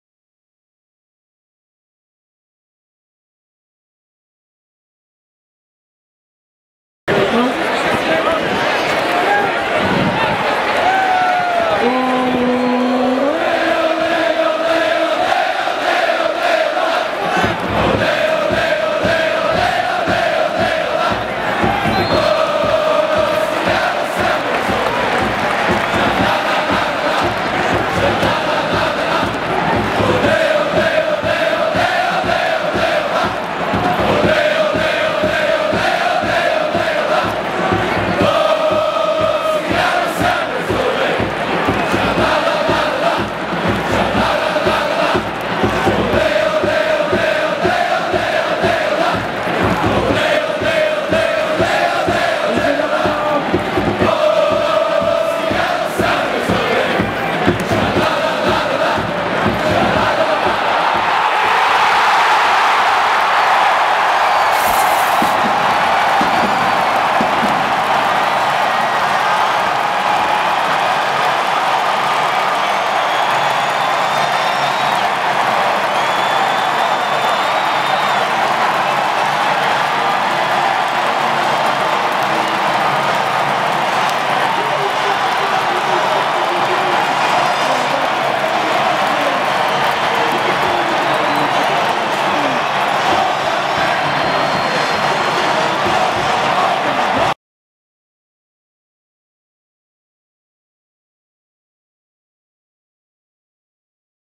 POGO with Oba Goal
A Pogo that is disrupted when Oba scores a goal for the Sounders!